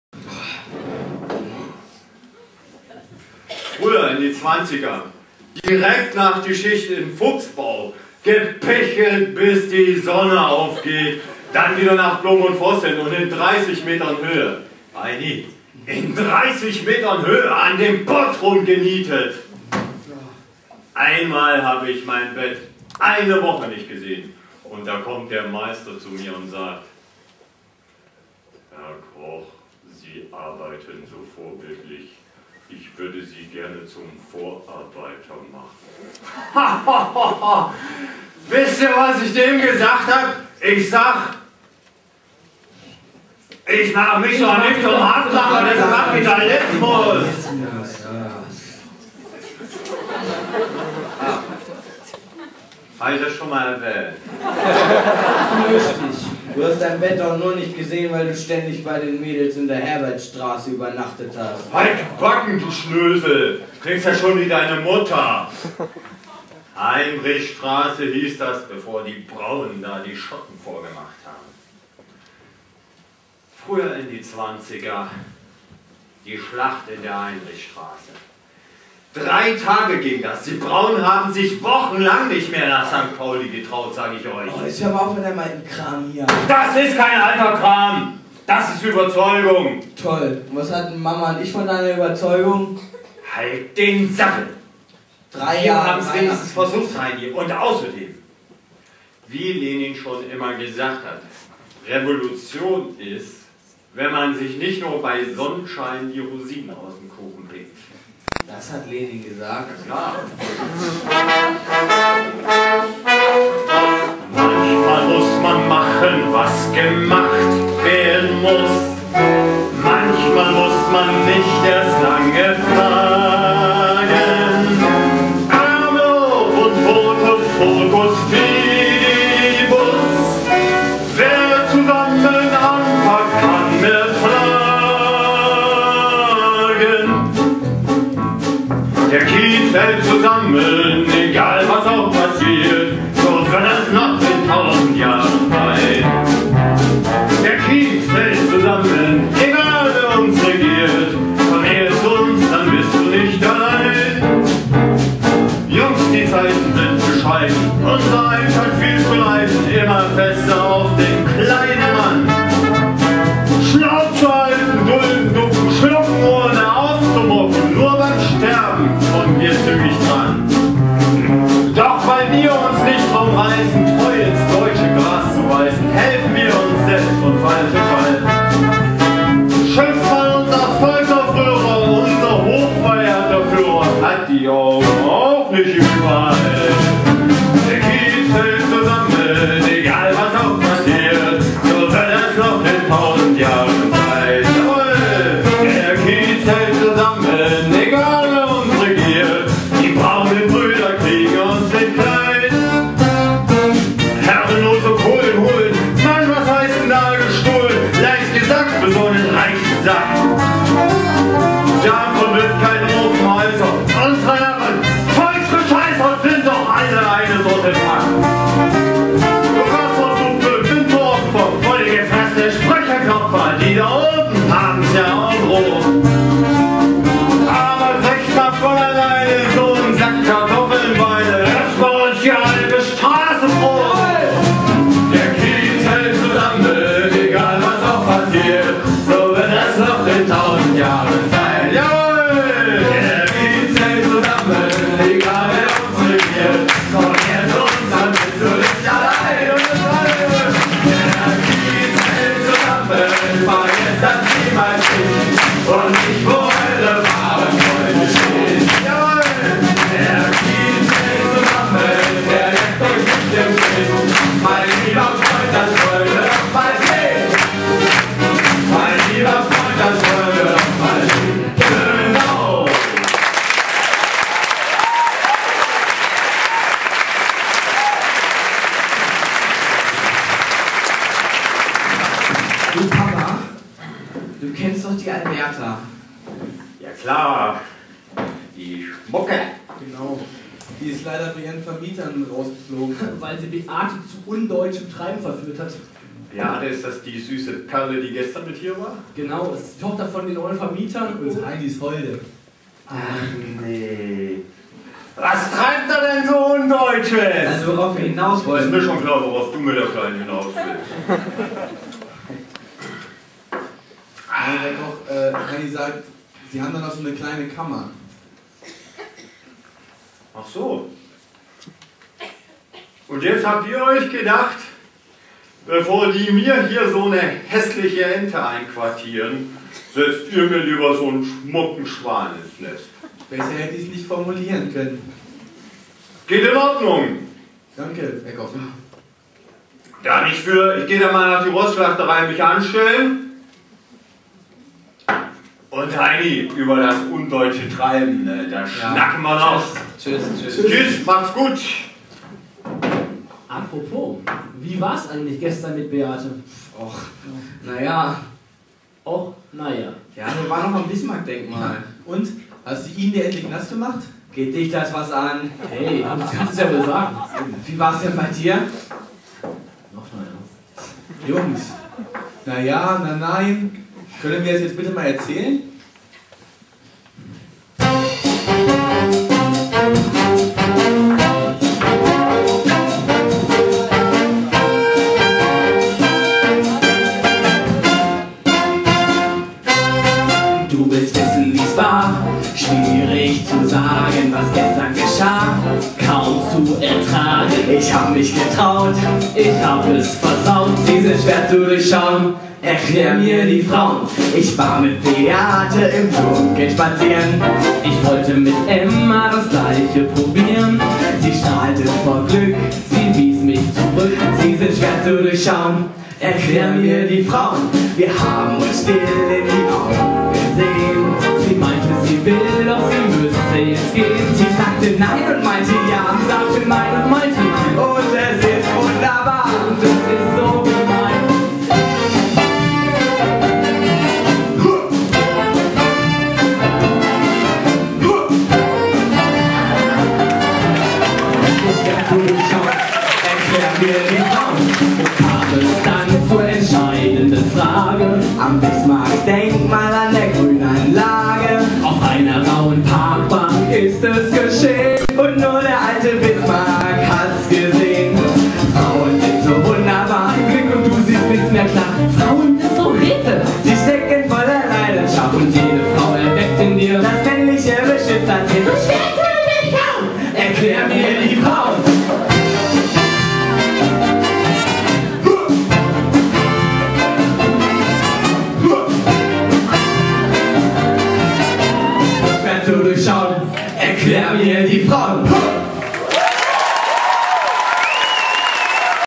Swinging St. Pauli - Teil 8
Die Aufnahme der Swinging St. Pauli Aufführung des Geschwister-Scholl-Gymnasiums Berenbostel vom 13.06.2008.

Achter Teil von vierzehn.